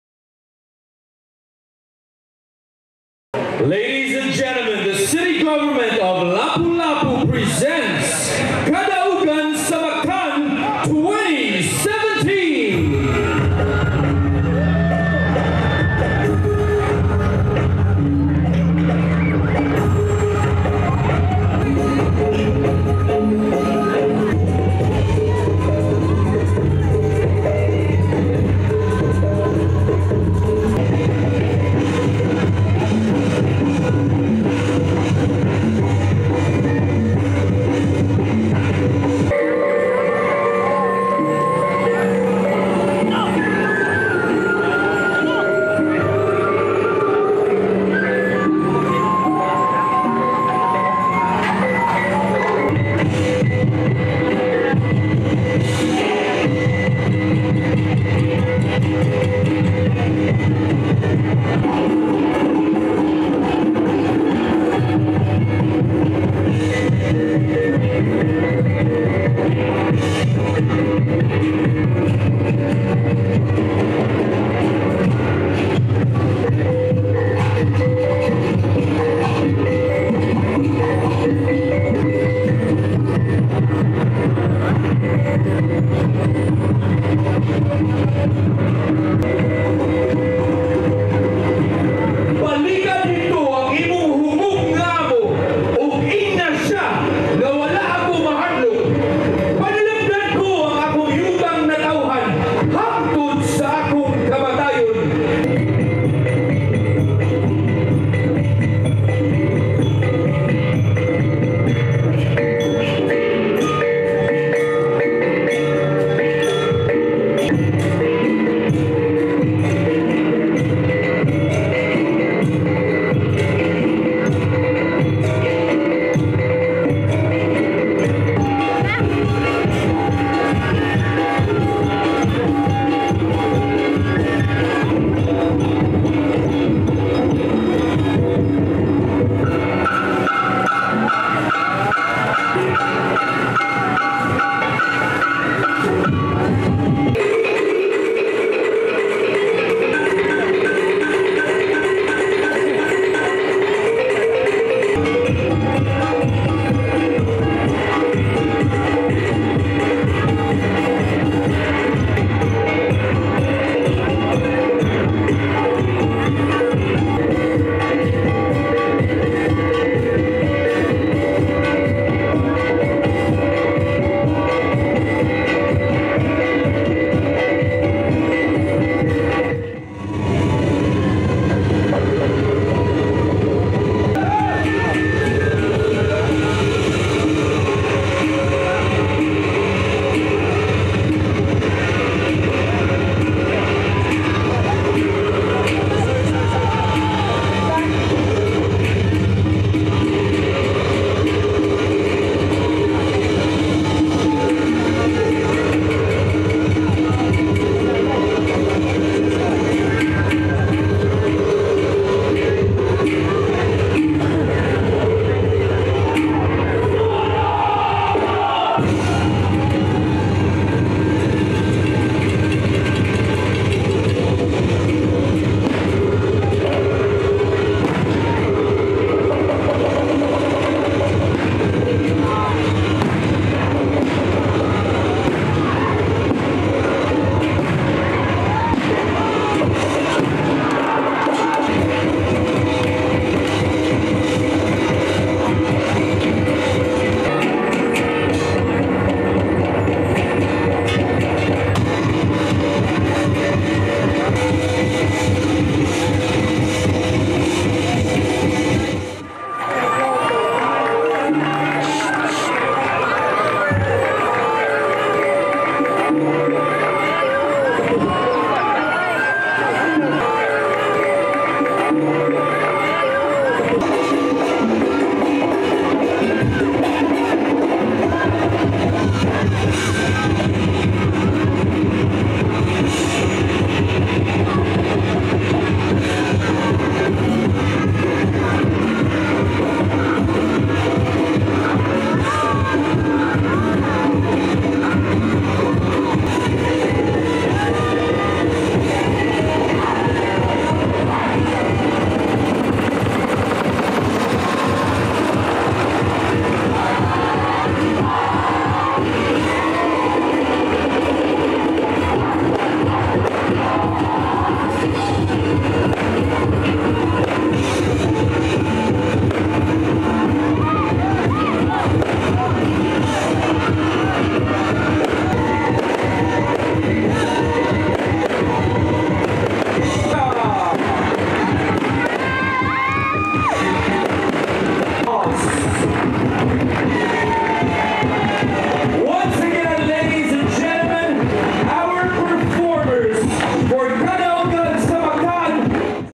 Kadaugan sa Mactan 2017
CEBU. Cebuanos commemorated on Thursday, April 27, the victory at the Battle of Mactan (Kadaugan sa Mactan) at the Liberty Shrine, Lapu-Lapu City. (Ojharra Manlosa/SunStar Philippines)